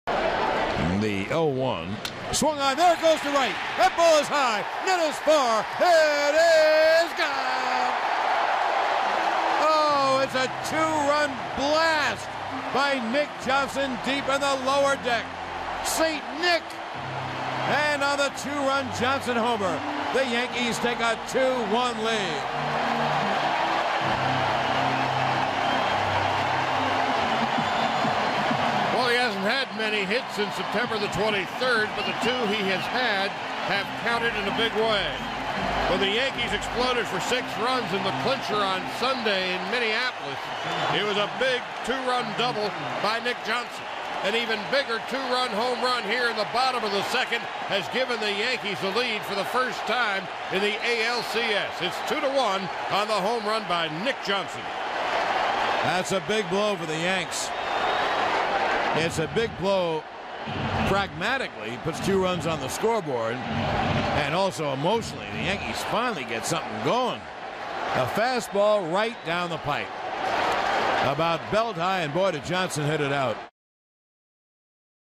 Johnson's homer gives Yankees lead in 2003 ALCS Gm 2
10/9/03: Nick Johnson hits a two-run home run to give the Yankees a 2-1 lead in the bottom of the 2nd of Game 2